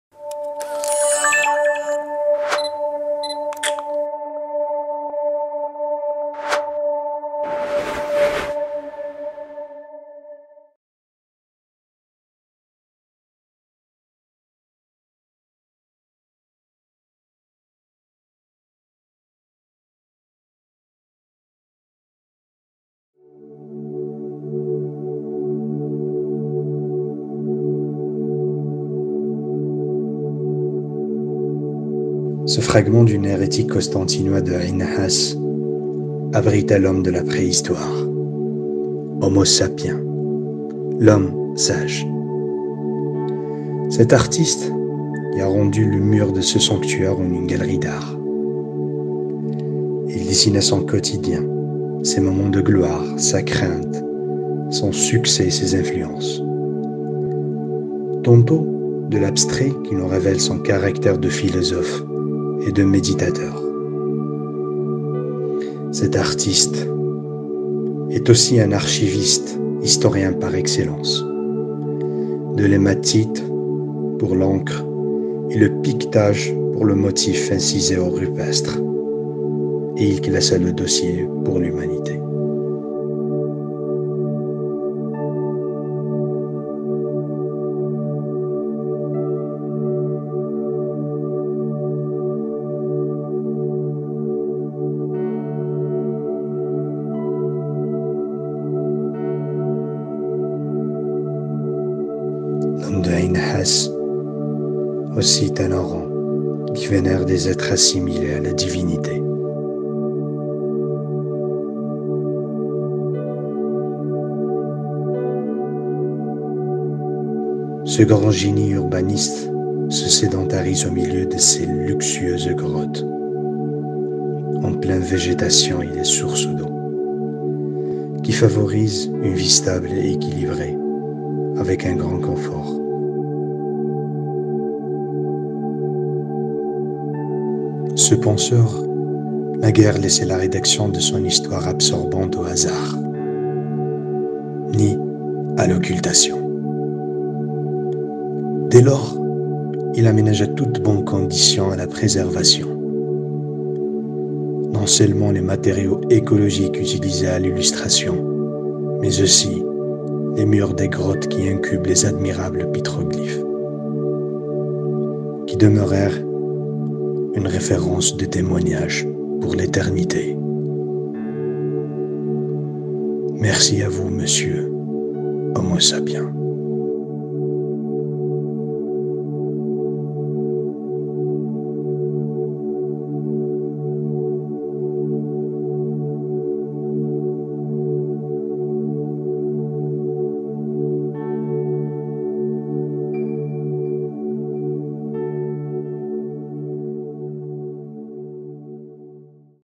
HOMINVM LEGATVM - Short Lecture
HOMINVM LEGATVM is a short philosophical  lecture in French of roughly 3 minutes. It offers a visceral and immersive look into the early Aboriginal rock paintings and cave-dwellings in Ain Nahas in Constantine. It is one of the most thought provoking visual remnants of prehistoric rock art that is  ever present for thousands of years in North Africa, showcasing the ancient animals like the Barbary sheep (Ammotragus lervia), as well as human creativity that tend to conjure images of mysterious figures.

Filmed and edited : Billel Benguedouar
Text and narration : Jacob Bou Arroudj